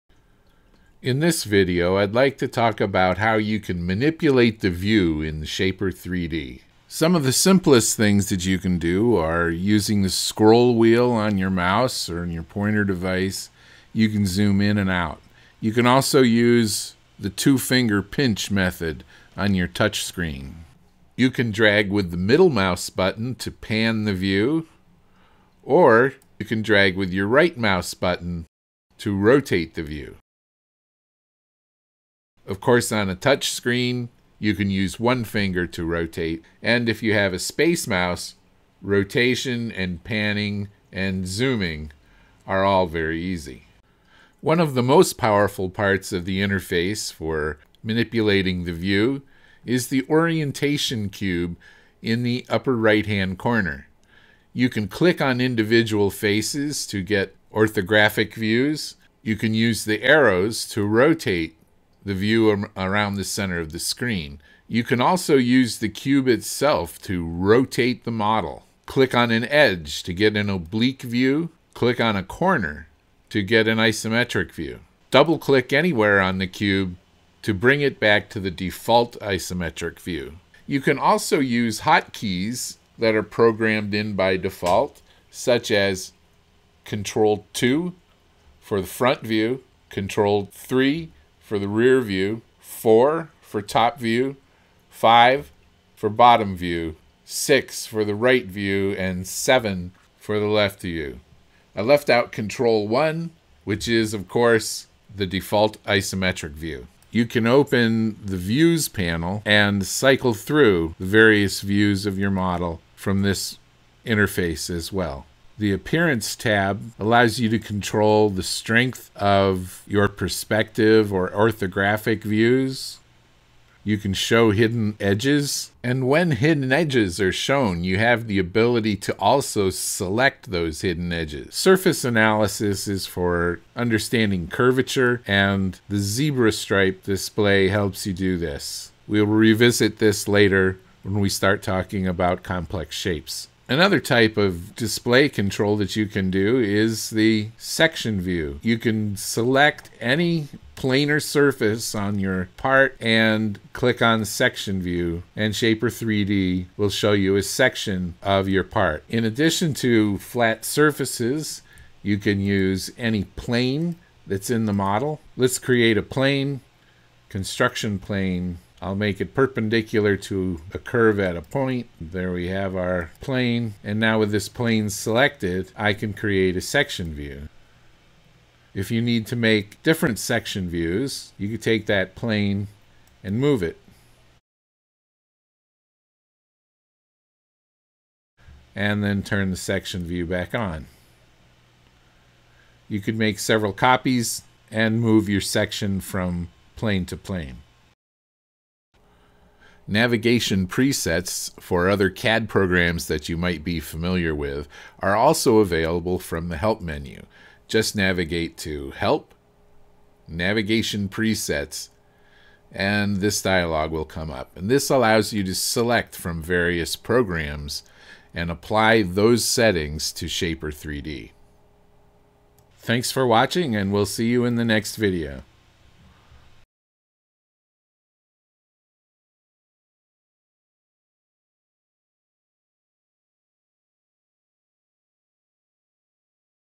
Control the view of your modeling environment | Shapr3D Basics
Explore how to move around your modeling space. You get best practices for your chosen input devices, go-to keyboard shortcuts, selecting navigation presets, and working with navigation islands for section views, construction planes, and more.